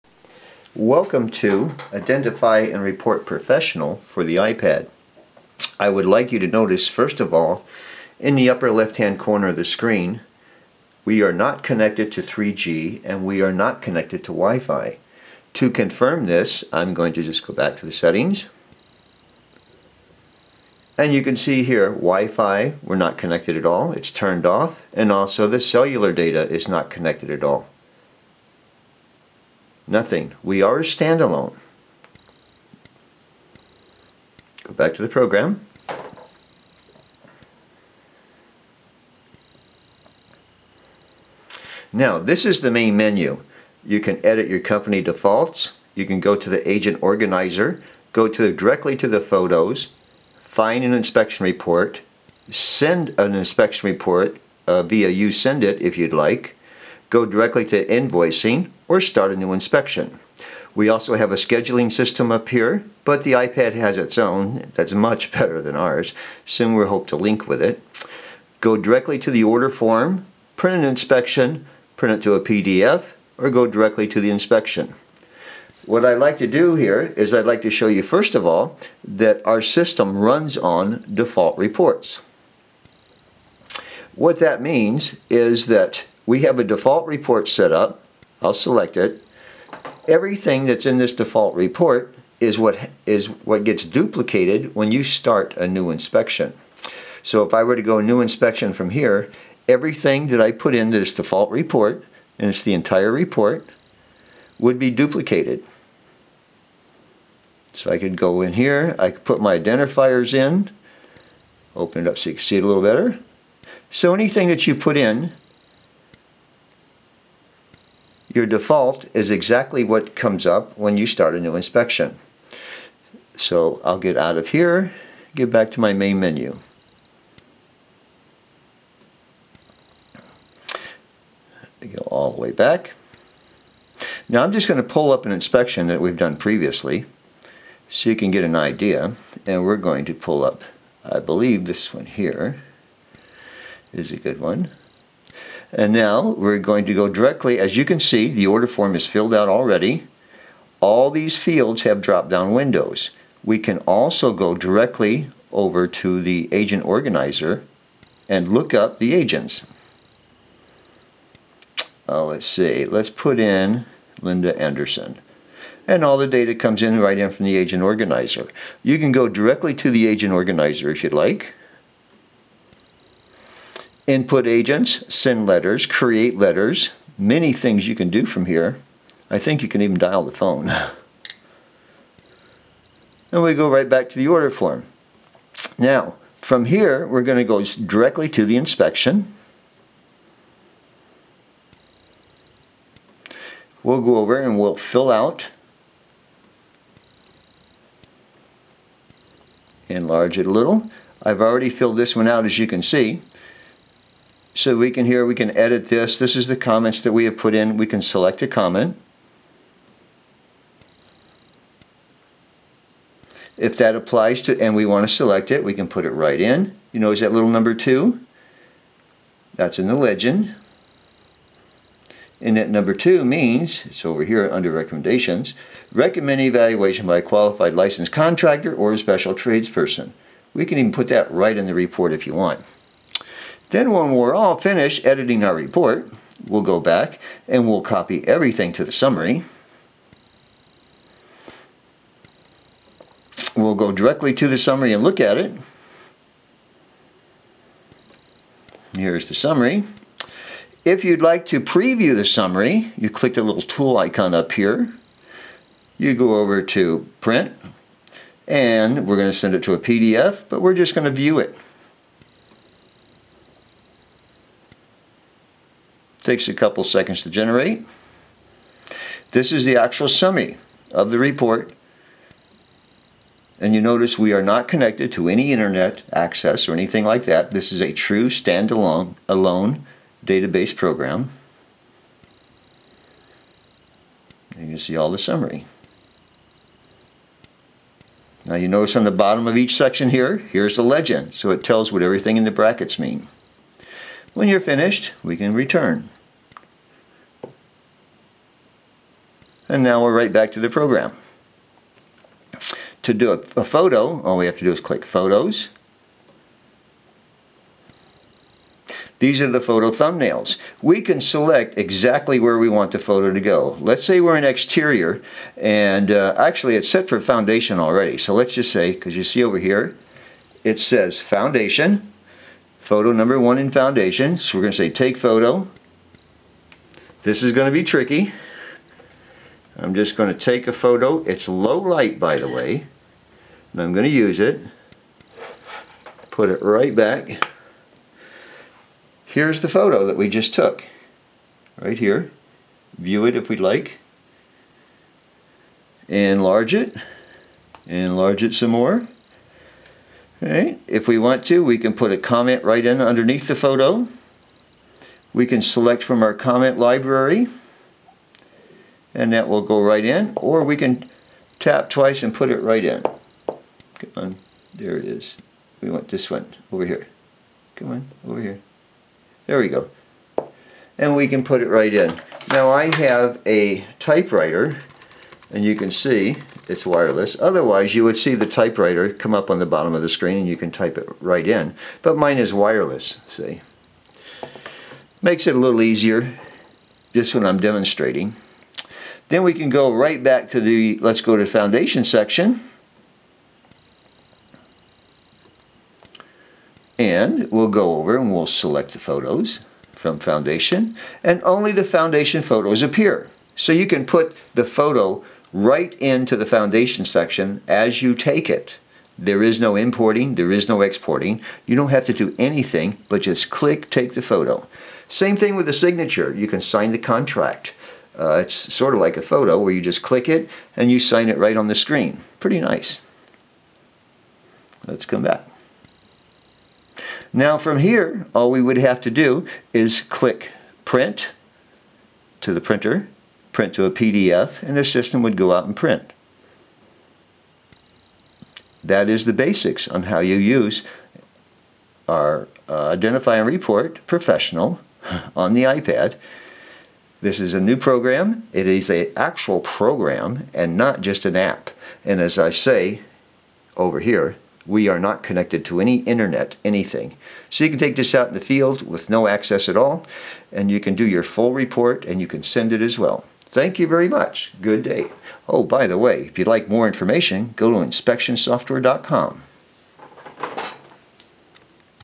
iPad Home Inspection Software Identify & Report Professional.MTS
A true iPad Home Inspection Program, Not another iPad App.  Identify & Report Professional was written to run on all platforms, Mac, Pc, and iPad.    Yes it's the same Software installed on any Platform and it runs as a stand alone Inspection Program on the iPad.
iPad Home Inspection Software / apps.  Now you can have the Same Property Inspection Software on your iPad, Mac and PC at the same time.   NO NEED to Deal with photos.   Take your Photo with the iPad and the Photo will be Automatically  inserted into the Report.    NO  Internet or WiFi  Access Required,  Identify & Report Professional is a Stand-Alone on the iPad.
Take your Photo with your iPad and it weill be automatically inserted into your Inspection Report..
Identify & Report Professional and the iPad Save You Time, It's That Simple.

Save Time and $$$$$$$$$$$$$